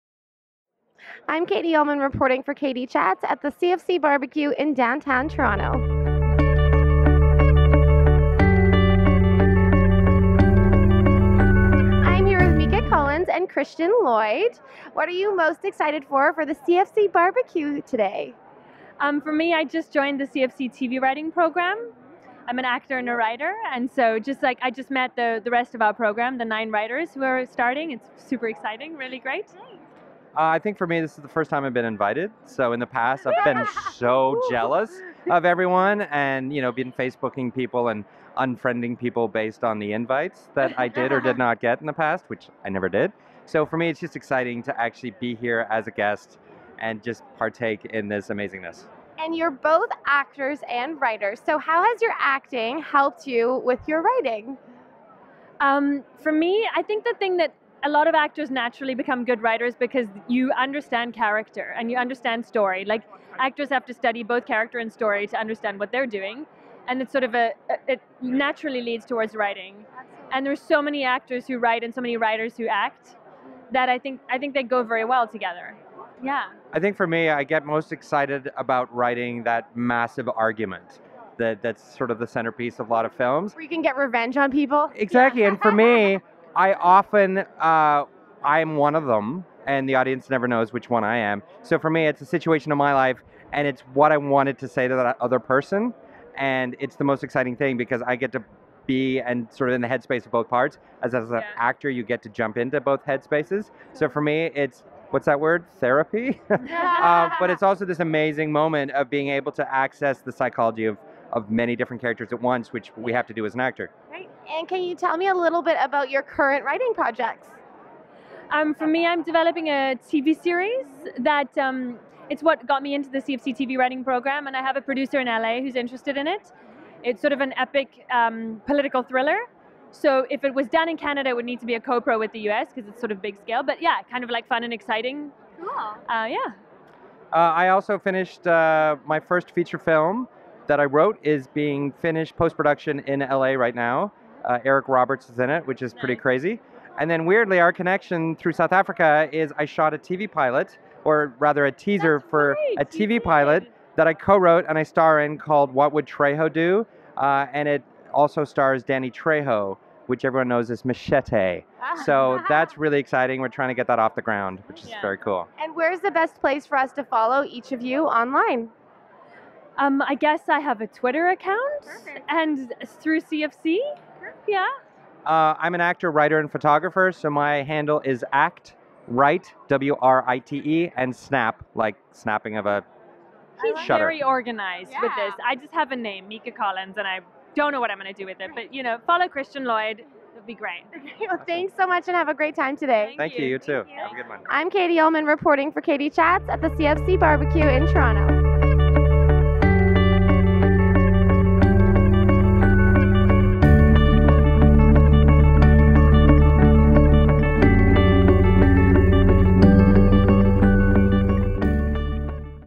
KATIE CHATS: CFC, MIKA COLLINS & CHRISTIAN LLOYD, FILMMAKERS/ACTORS
KATIE CHATS with Filmmakers/Actors Christian Lloyd & Mika Collins at the CFC BBQ during tiff 2014 (TORONTO INTERNATIONAL FILM FESTIVAL)

THANK YOU TO:
Hairstylist/Makeup Artist
Oriana Rossi
Jewellery Designer
Stella Margaritis

Christian, April 21
Mika, July 30